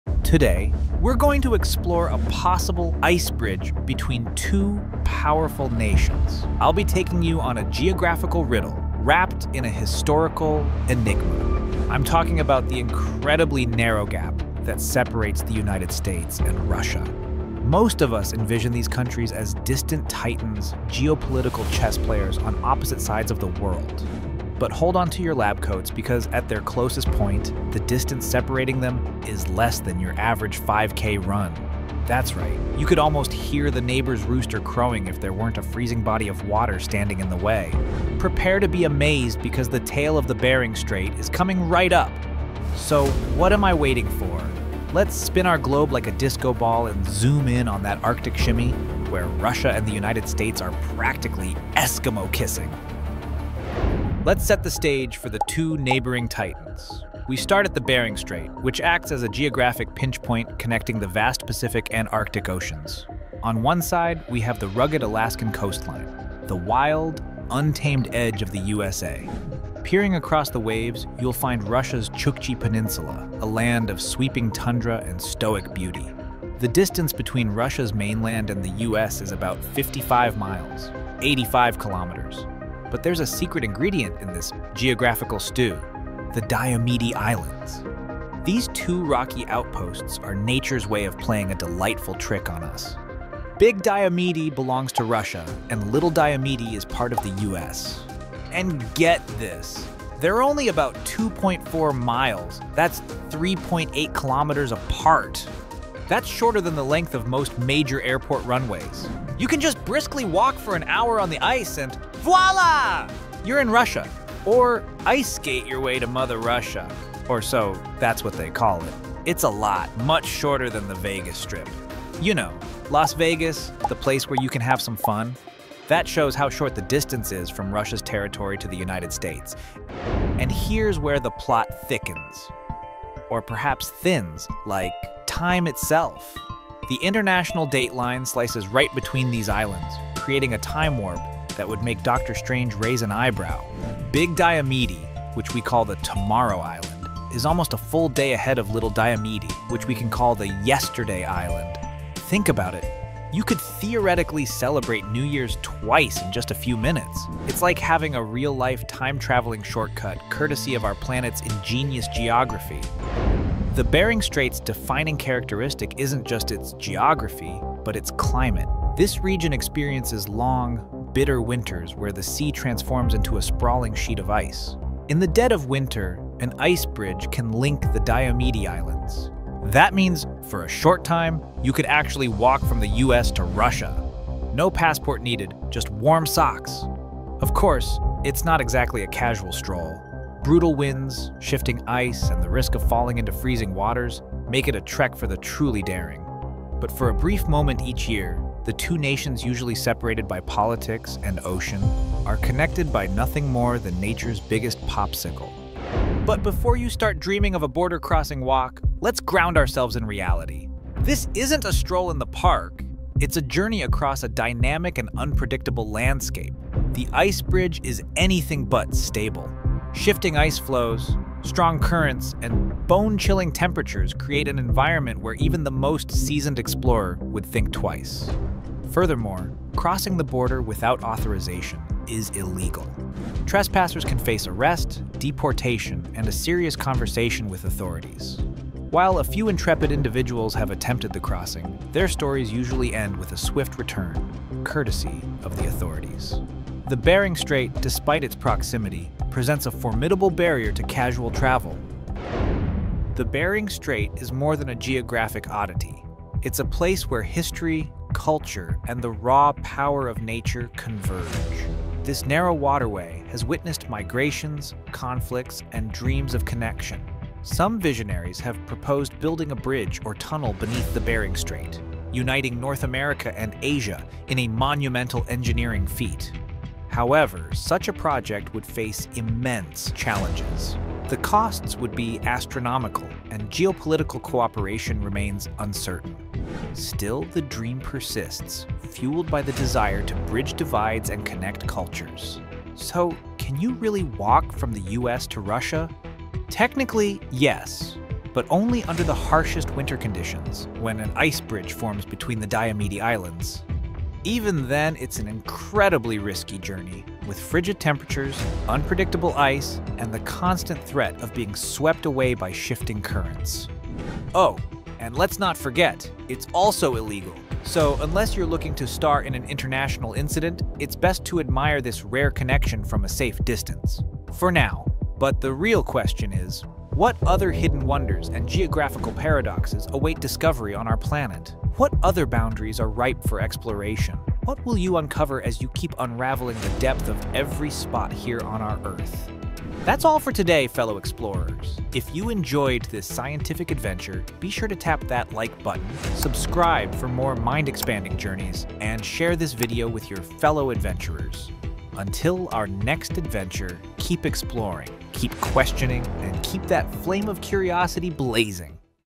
The Hidden Bridge Between USA and Russia That Will Blow Your Mind!
Strap on your snow boots and grab your curiosity—today we're venturing to one of Earth's chilliest and quirkiest locations!
Join your favorite science-loving professor as we unravel the icy enigma of the Bering Strait: the astonishingly narrow gap between the USA and Russia. From frosty footpaths across frozen seas to time-traveling islands split by the International Date Line, this adventure is packed with jaw-dropping geography and frozen oddities.
Can you really walk from America to Russia? Why are two islands just 2.4 miles apart nearly a day apart in time?

Let's find out—as we keep unraveling the depths of every spot on our Earth!